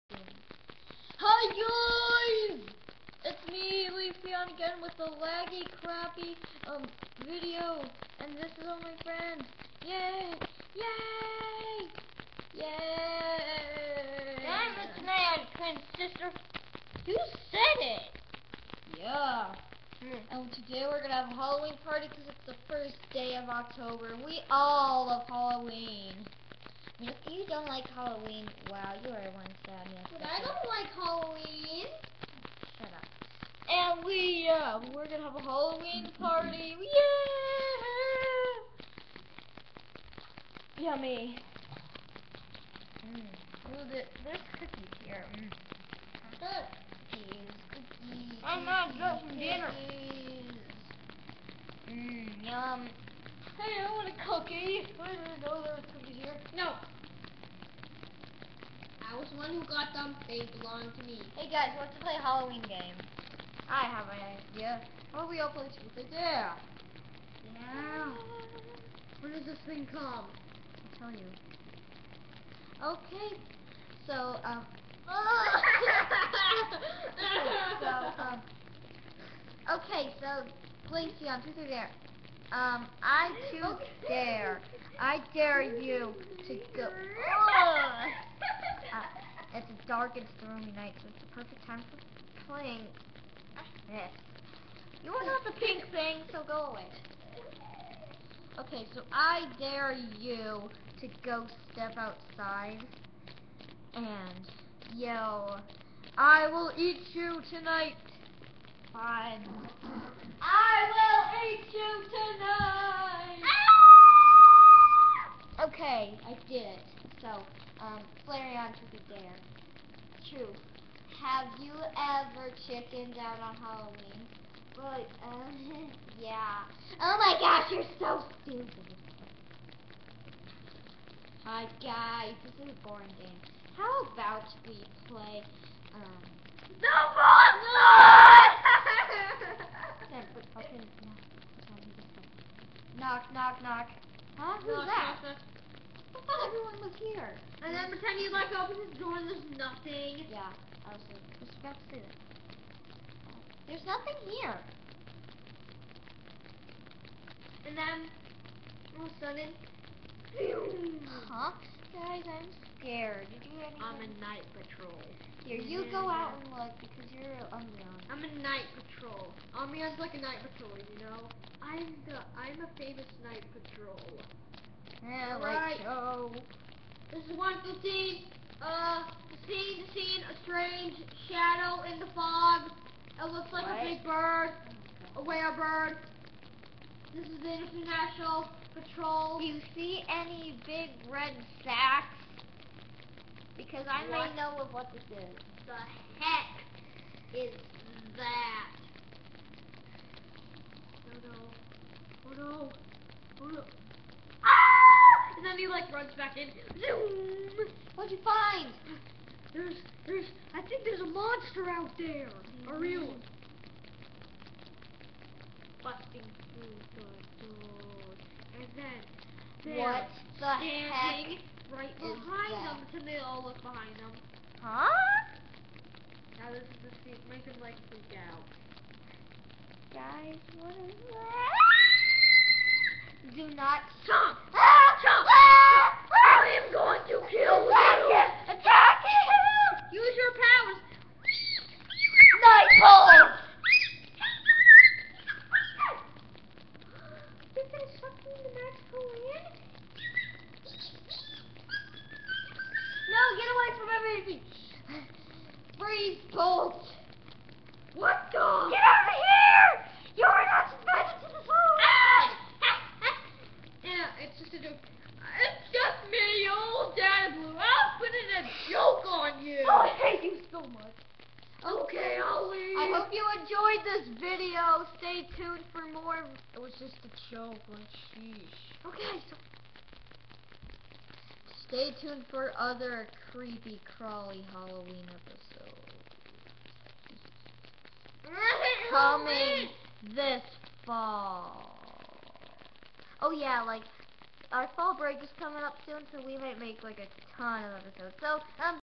EEVEE HALLOWEEN PARTY!!
They eevees have a halloween party but just when a strange vistor comes to the door.